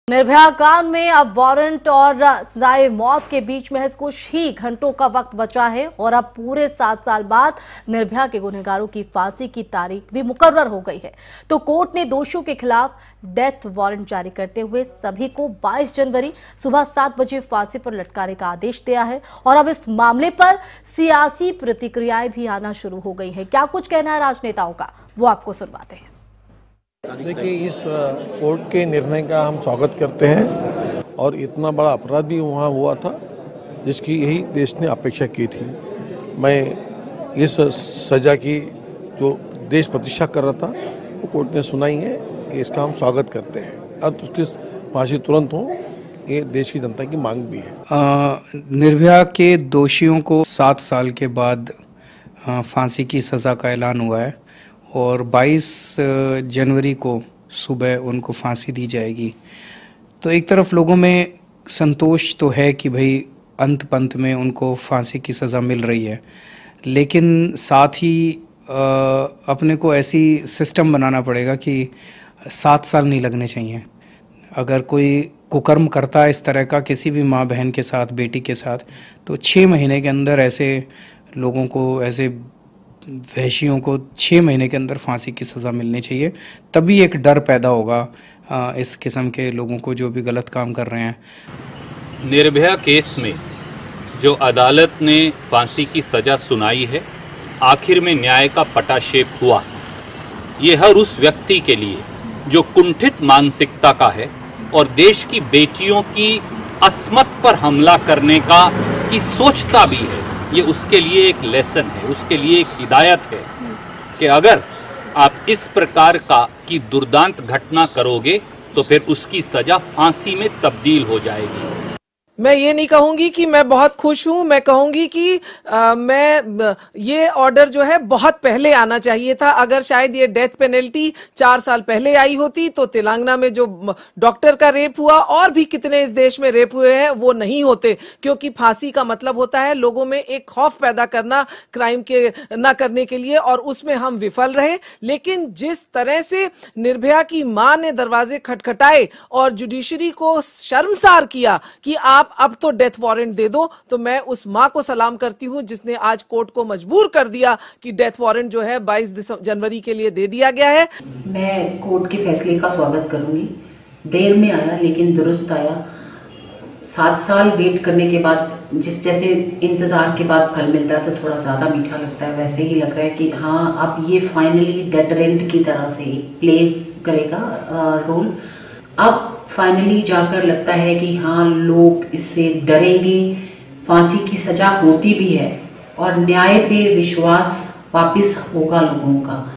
निर्भया के दोषियों को फांसी के आदेश पर राजनीतिक दलों ने दी प्रतिक्रिया, जानें क्याा कहा
आखिरकार पूरे सात साल बाद निर्भया के गुनहगारों की फांसी की तारीख मुकर्रर हुई है... कोर्ट ने दोषियों के खिलाफ डेथ वारंट जारी करते हुए सभी को 22 जनवरी सुबह 7 बजे फांसी पर लटकाने का आदेश दिया है... अब इस मामले पर सियासी प्रतिक्रियाएं भी आनी शुरु हो गई हैं...